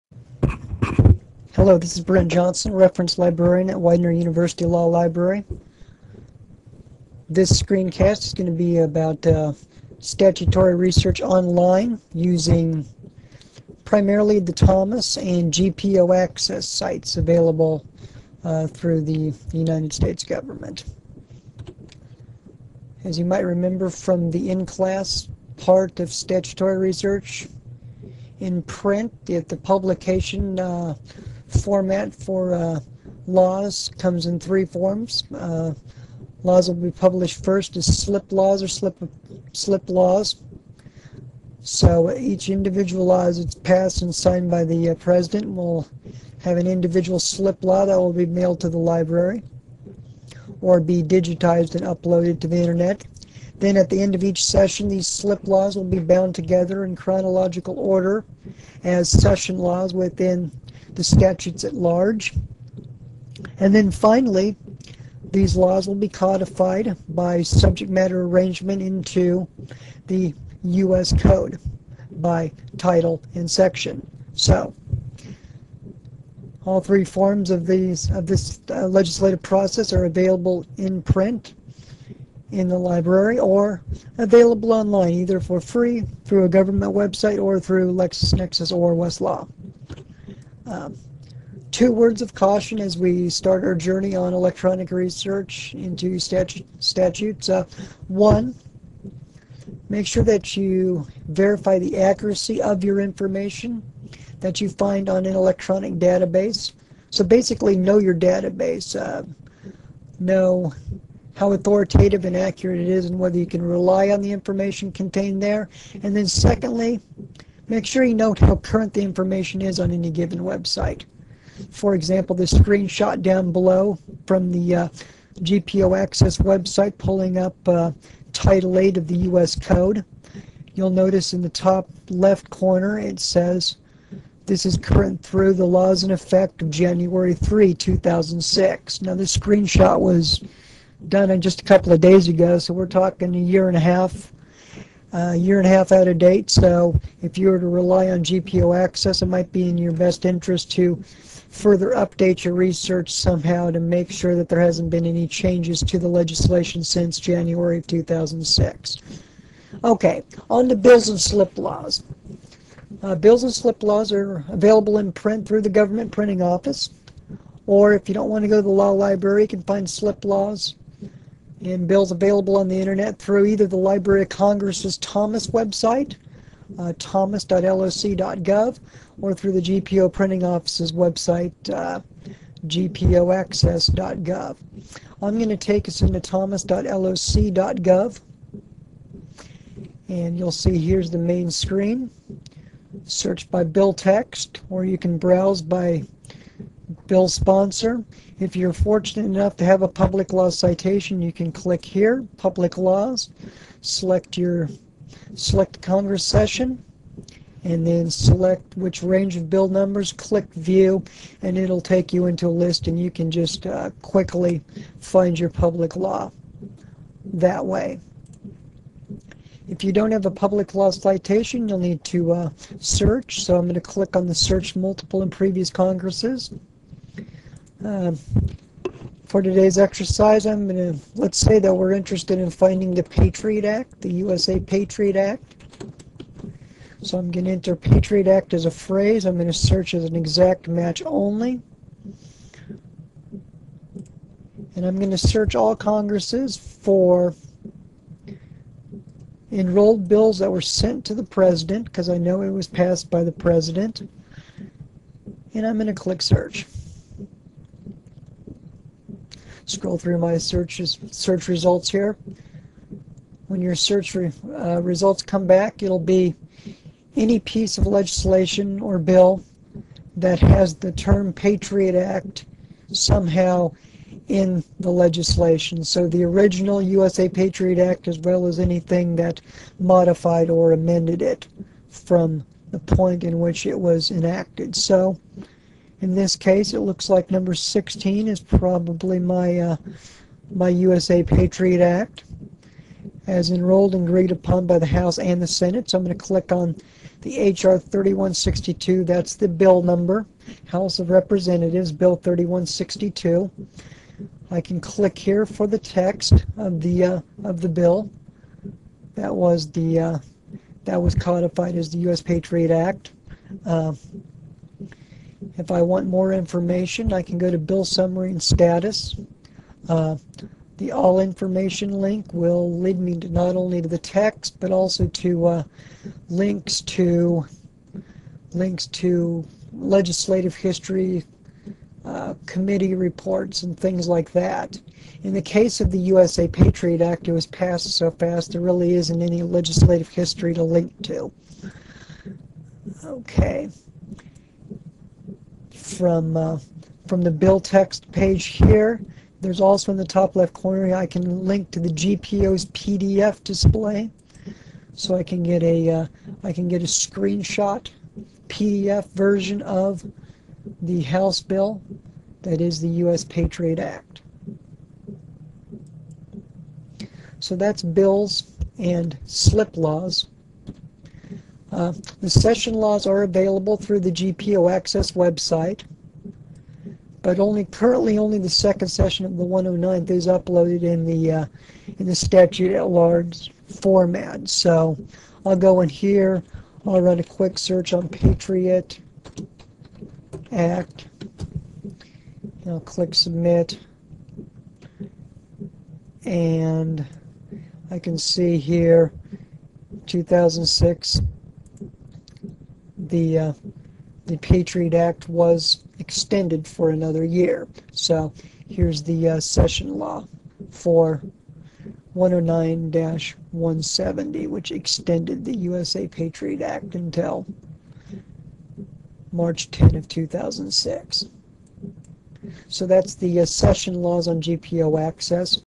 Statutory Research 1
Tutorial on using GPO Access and Thomas websites to find bills, slip laws, and session laws.